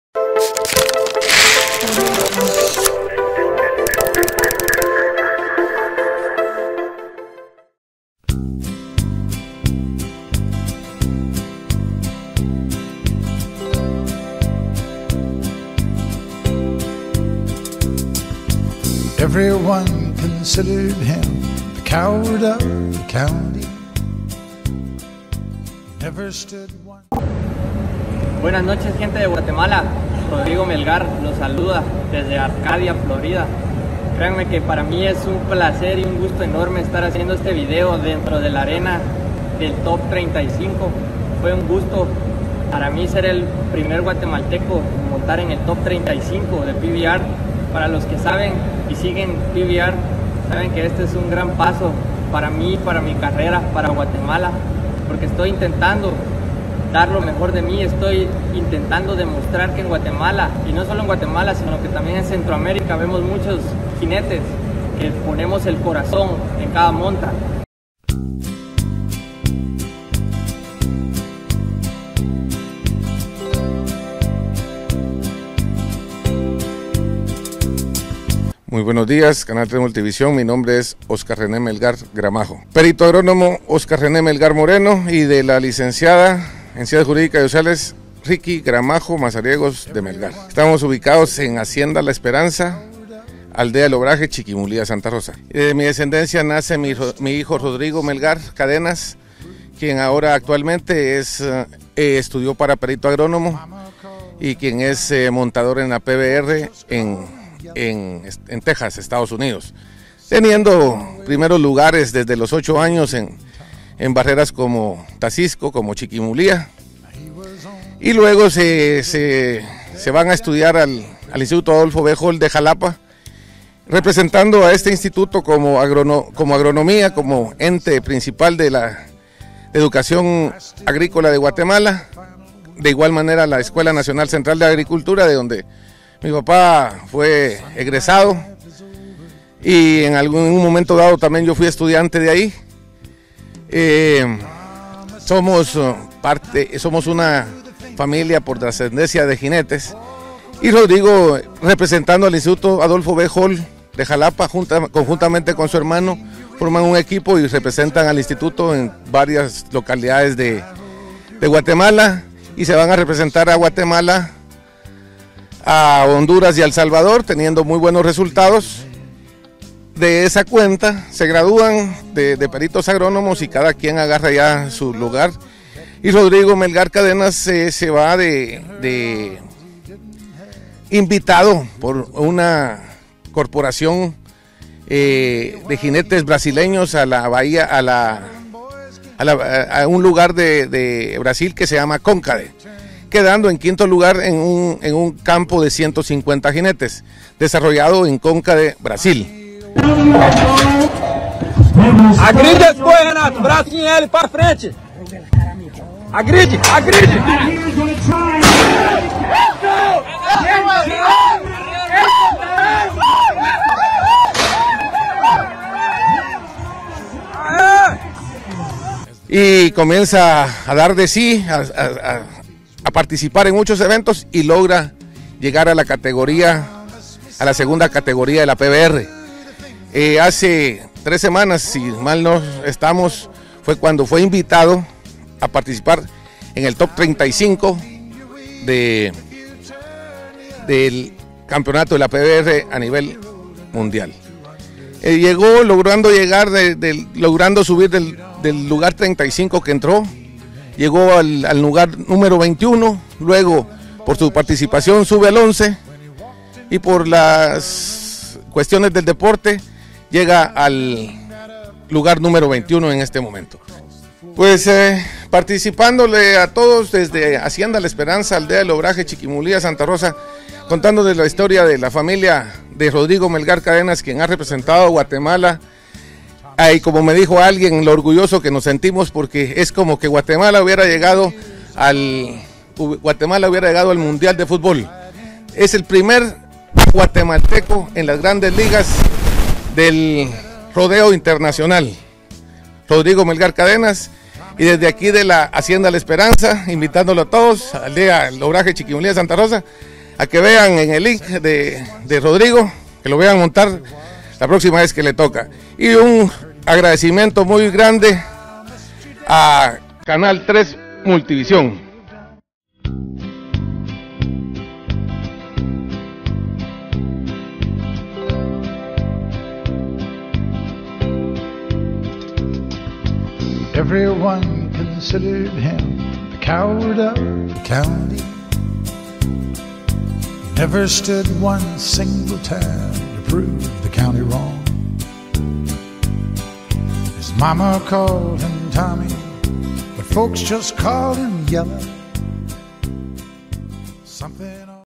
GUATEMALA participa en competencia de montas mundiales
Rodrigo Melgar Cadenas, representante de Guatemala con participación en competencia mundial de montas.